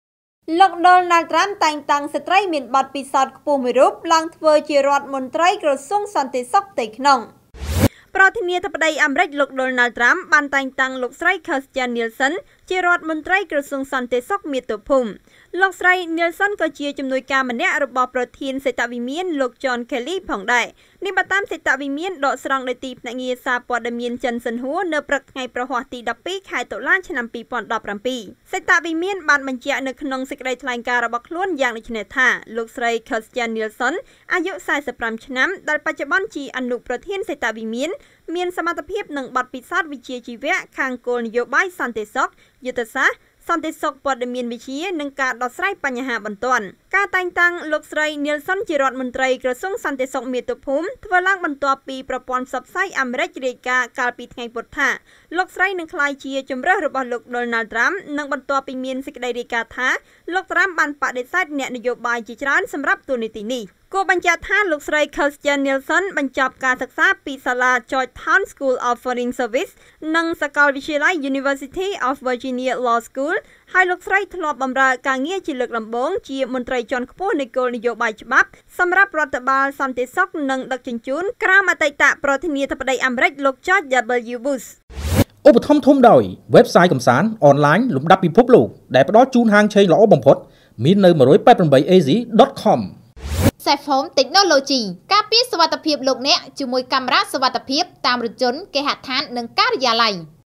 លោក ដូណាល់ ត្រាំ «តែងតាំងស្ត្រីមានបទពិសោធន៍ខ្ពស់មួយរូប ឡើងធ្វើជារដ្ឋមន្ត្រីក្រសួង...
លោក ដូណាល់ ត្រាំ «តែងតាំងស្ត្រីមានបទពិសោធន៍ខ្ពស់មួយរូប ឡើងធ្វើជារដ្ឋមន្ត្រីក្រសួងសន្តិសុខផ្ទៃក្នុង»